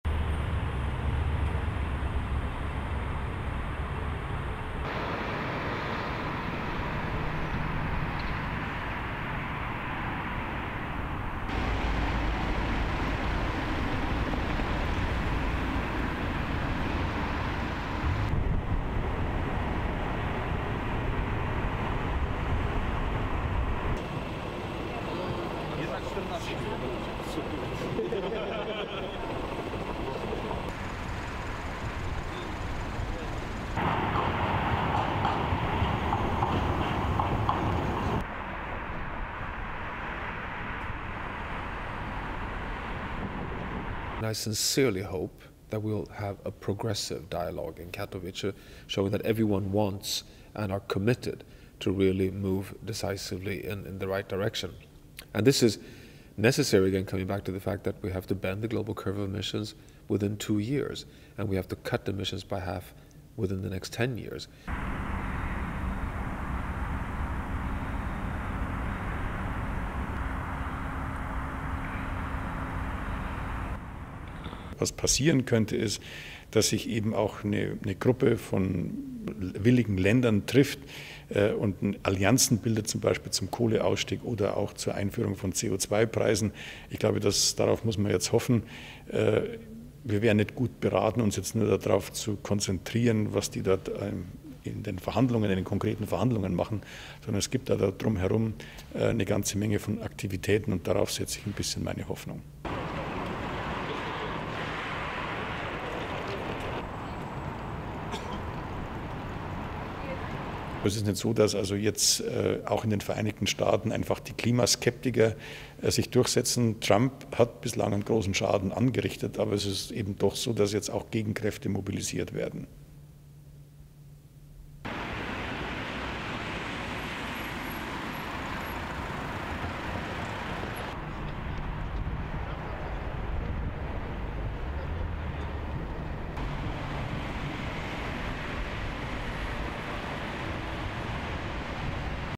Security measures high ahead of climate summit
(1 Dec 2018) Security measures are high as negotiators gathered in Katowice, Poland, for the annual UN climate summit, also known as COP24.
It is the most important international meeting for countries to discuss and coordinate the fight against global warming.
Three years after sealing a landmark global climate deal in Paris, world leaders are gathering again to agree on the fine print.
The euphoria of 2015 has given way to sober realisation that getting an agreement between almost 200 countries, each with their own political and economic demands, would be challenging, as evidenced by President Donald Trump's decision to pull the United States out of the Paris accord, citing his mantra "America First."
The Director and Chief Economist of the Potsdam Institute for Climate Impact Research (PIK), Ottmar Edenhofer, said that "Trump has caused great harm so far, but it is also the case that opposing forces are mobilised."
Top of the agenda will be finalising the so-called Paris rulebook, which determines how countries have to count their greenhouse gas emissions, transparently report them to the rest of the world and reveal what they are doing to reduce them.
Seasoned negotiators are calling the meeting, which is expected to draw 25,000 participants, 'Paris 2.0' because of the high stakes at play in Katowice.
The event starts on Sunday and ends on December 14.